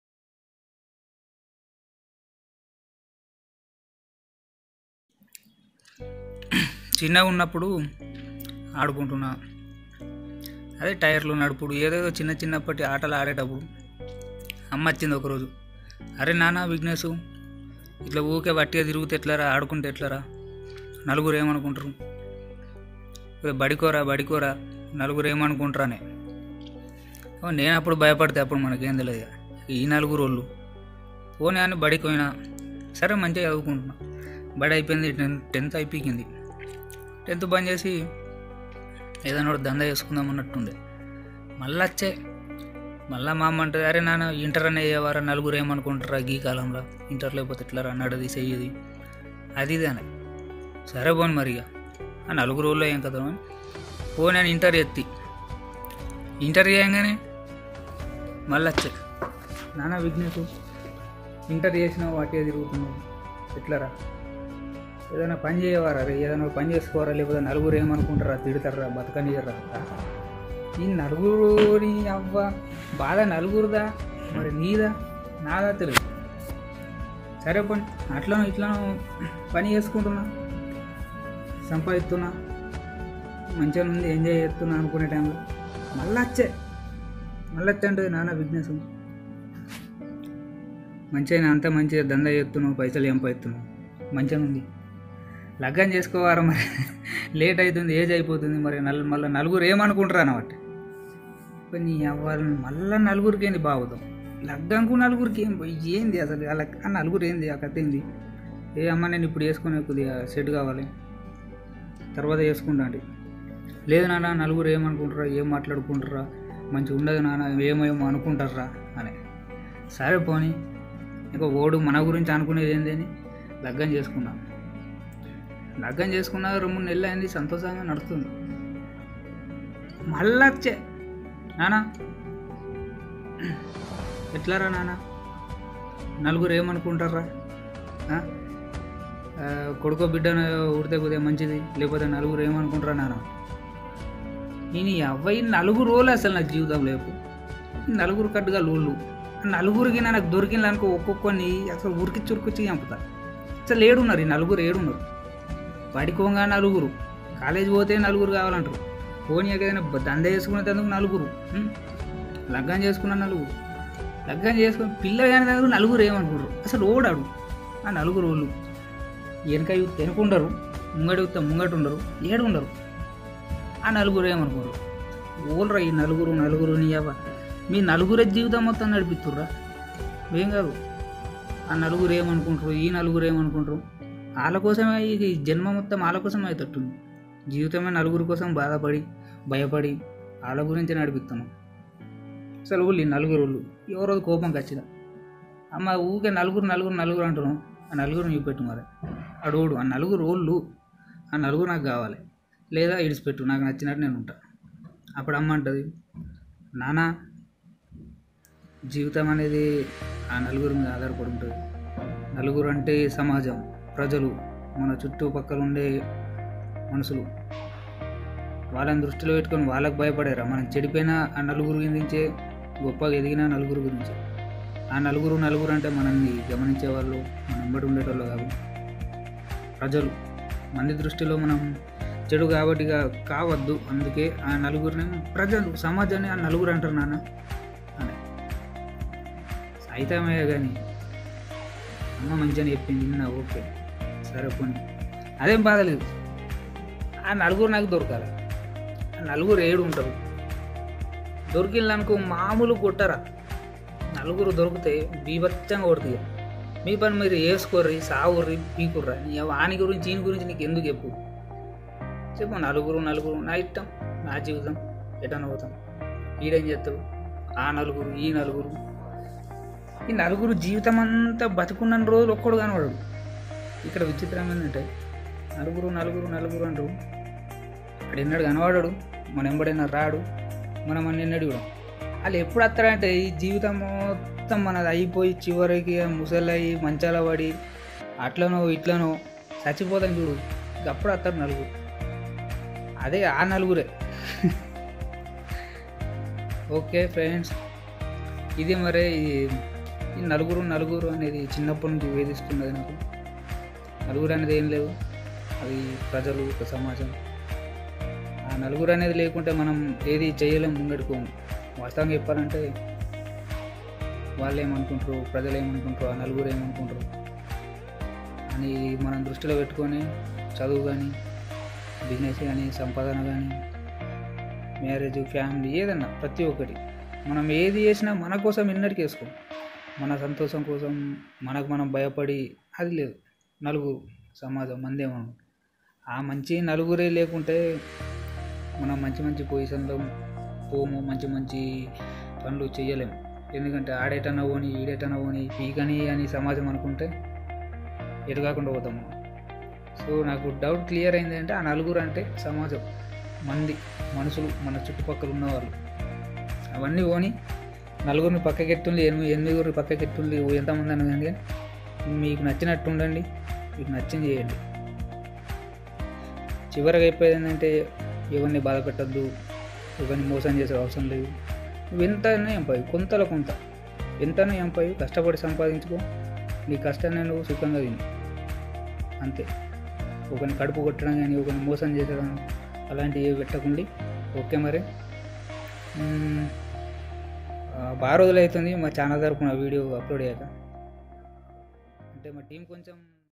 AAAA NALUGURU | ఆ నాలుగురు | తెలుగు | Vignesh Addict Vlogs
ఆ నాలుగురు మన లైఫ్ లో కచ్చితంగా ఉంటారు,

సొసైటీ మన మంచి చెడులు చేస్తూ ఉంటది,

what society said about our life,

social message